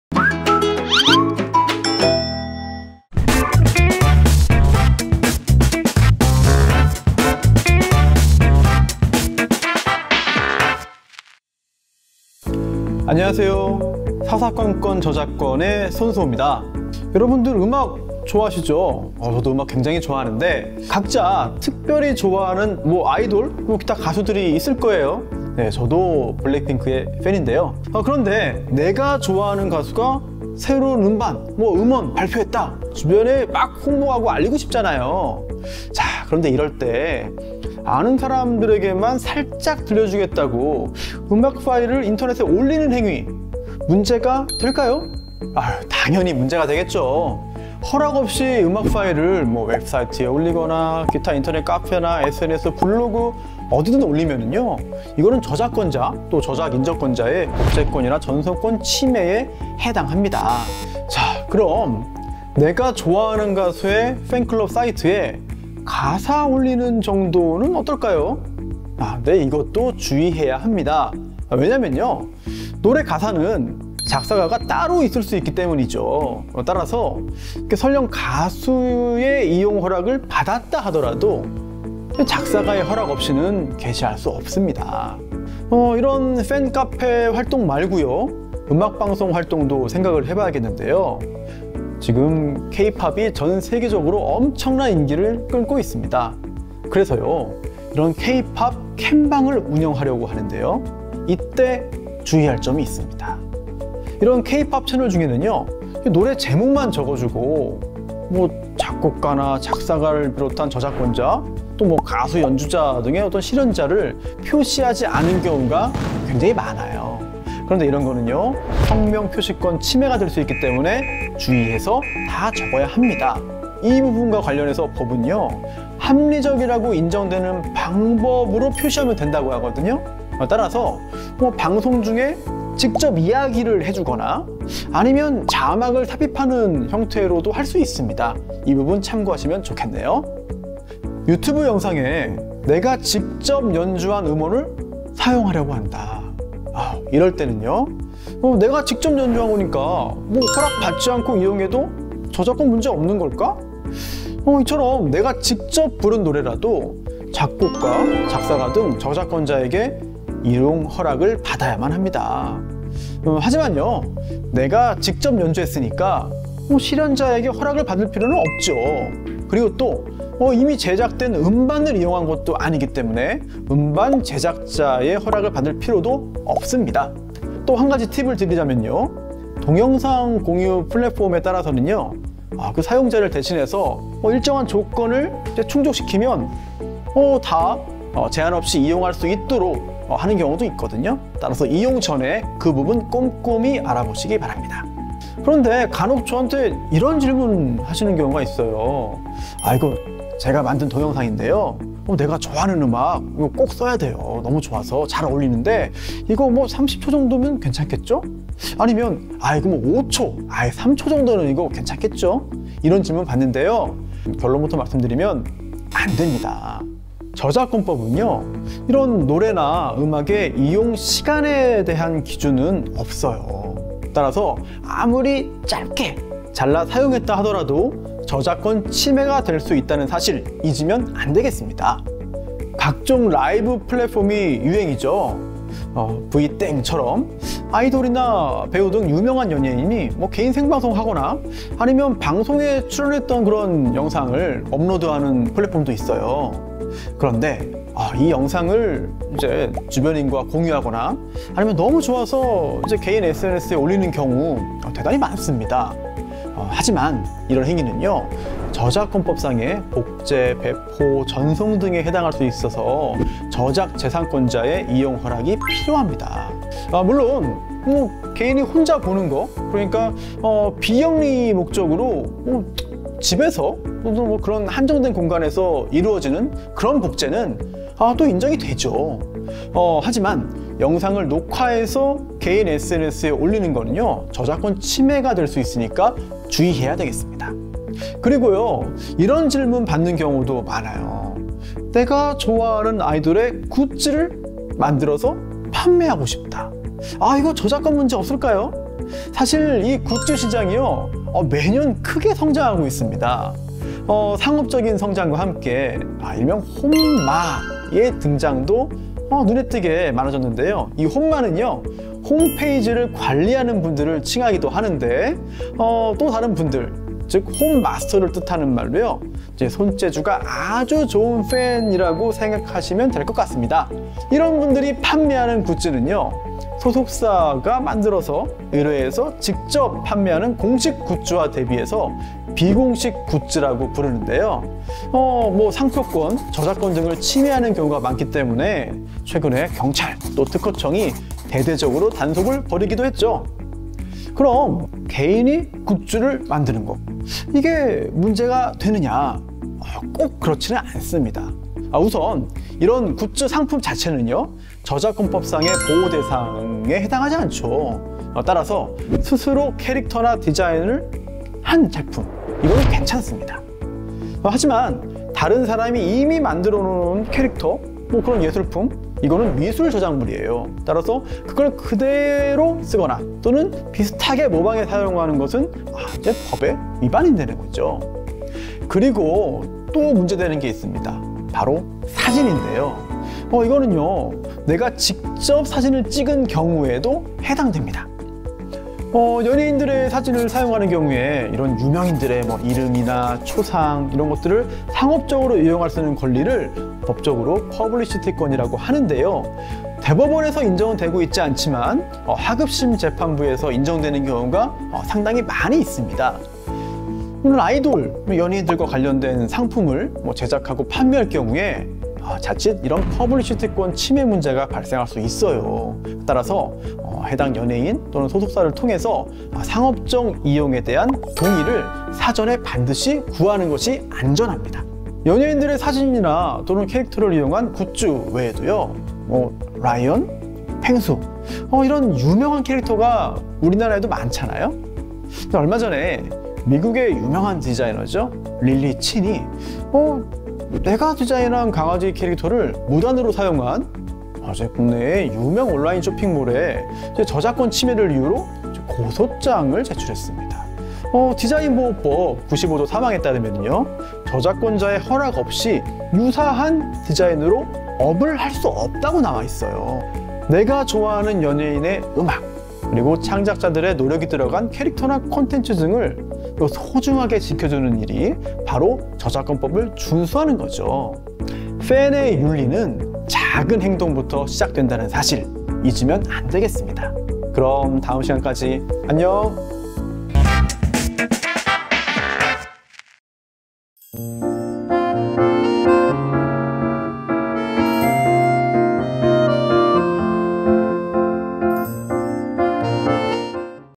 (SUB) 좋아하는 가수의 음악을 업로드 하고 굿즈를 만들고 싶다면 주목!
문제없이 좋아하는 가수의 음악을 홍보하고 굿즈를 만들고 싶다면?
지금 알아보자!

이 영상은 영문, 중문 자막을 제공하고 있습니다.
This video provides English and Chinese subtitles.
这段视频提供英文字幕和中文字幕。